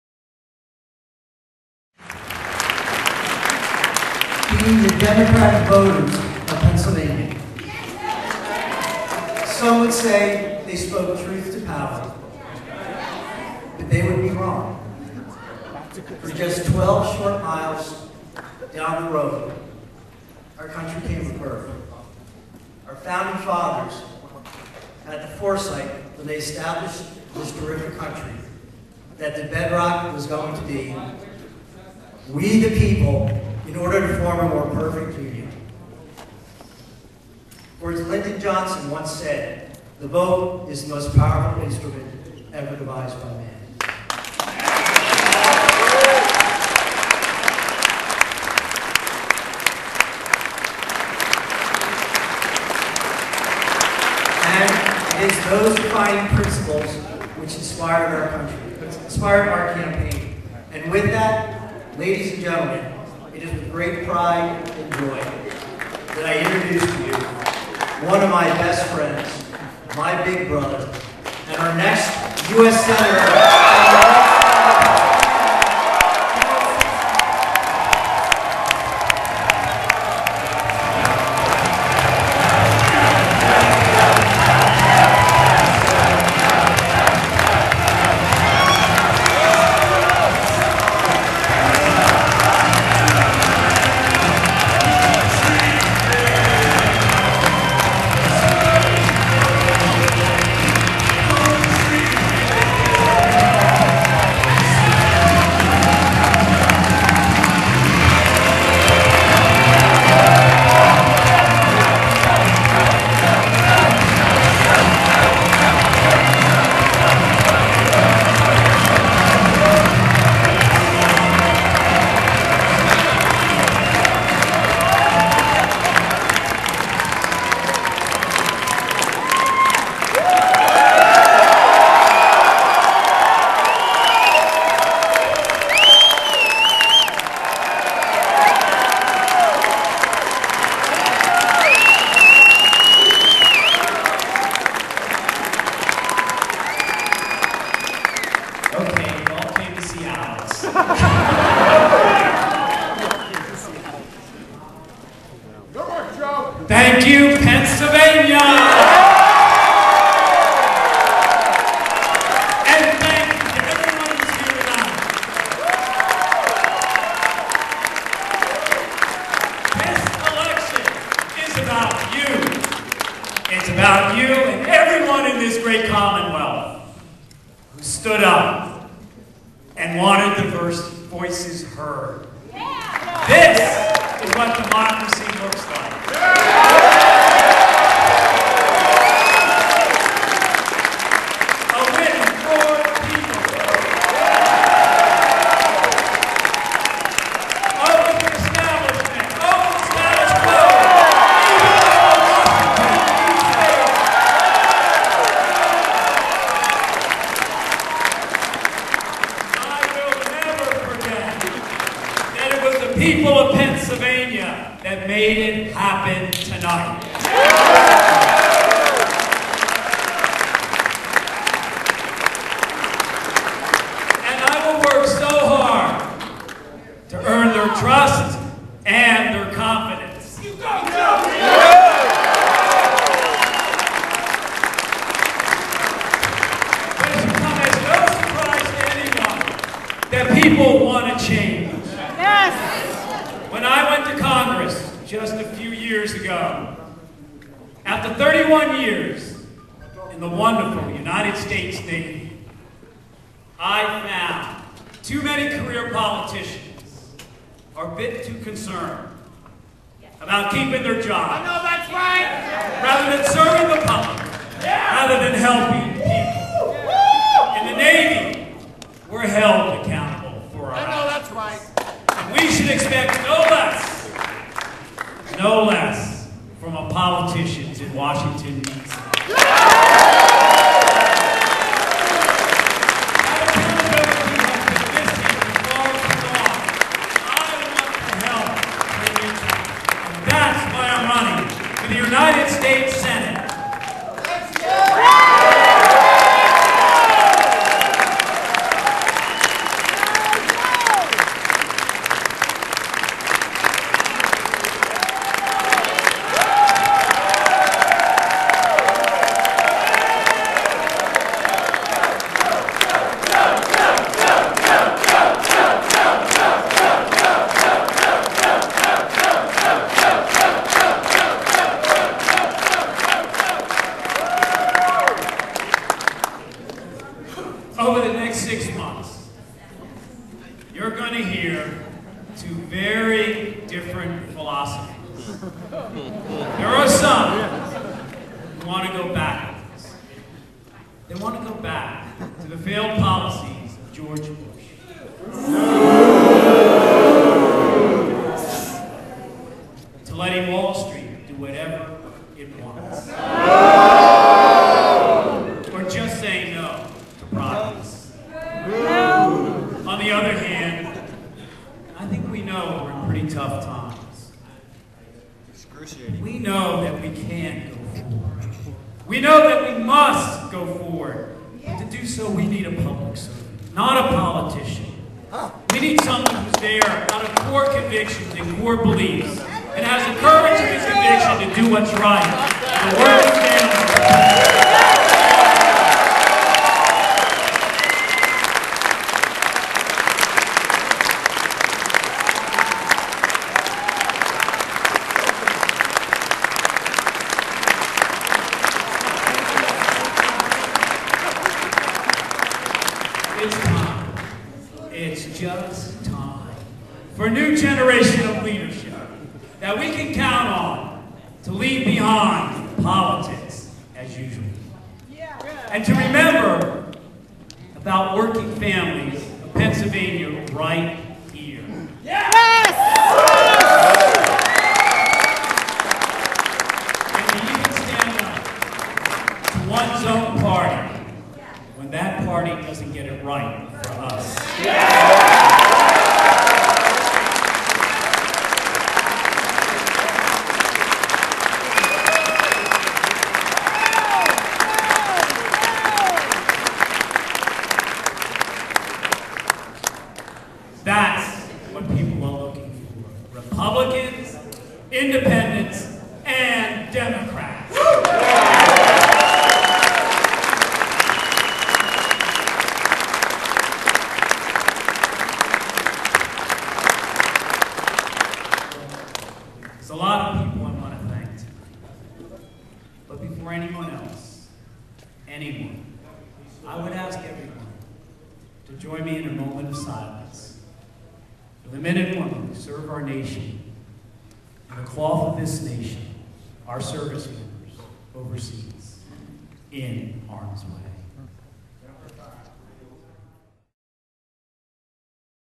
Sestak Victory Speech Part 1
Joe Sestak celebrates his victory over Arlen Specter.